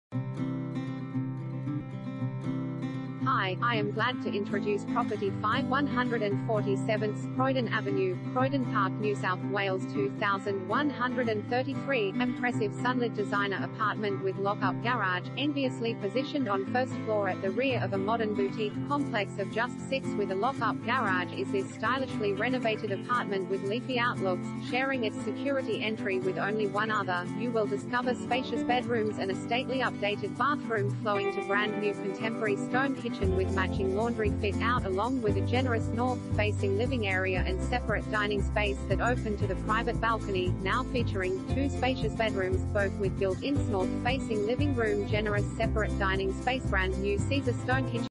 5/147 Croydon Avenue, Croydon Park NSW 2133 | AuPropertyReport.Com
5/147 Croydon Avenue, Croydon Park NSW 2133
Property Type: apartment
Bedrooms: 2
Bathrooms: 2
Car Space: 1
Impressive Sunlit Designer Apartment With Lock Up Garage!